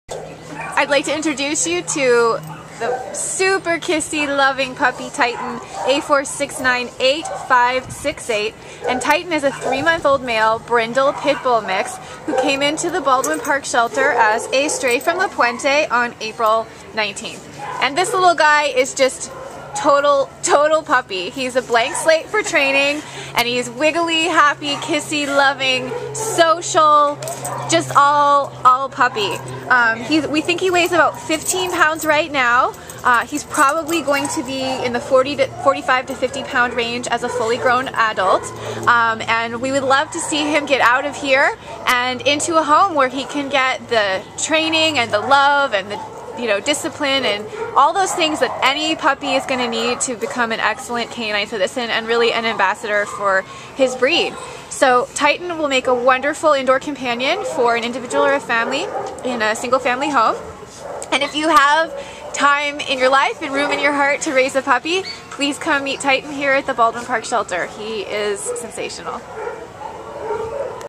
A4698568 Titan
A4698568  Titan is a vibrant 3-month-old brindle male Pit Bull mix puppy who was found in La Puente and brought to the Baldwin Park Animal Care Center on April 19th. Weighing 15 lbs now and likely to weigh 45--50 pounds fully grown, Titan is a wiggly and kissy boy with the cutest, chubbiest belly in town! Attentive and eager to please, he will surely take to his puppy training classes with excitement and ease. Titan will make a wonderfully playful and cuddly indoor pet for absolutely any individual or family living in a single-family home.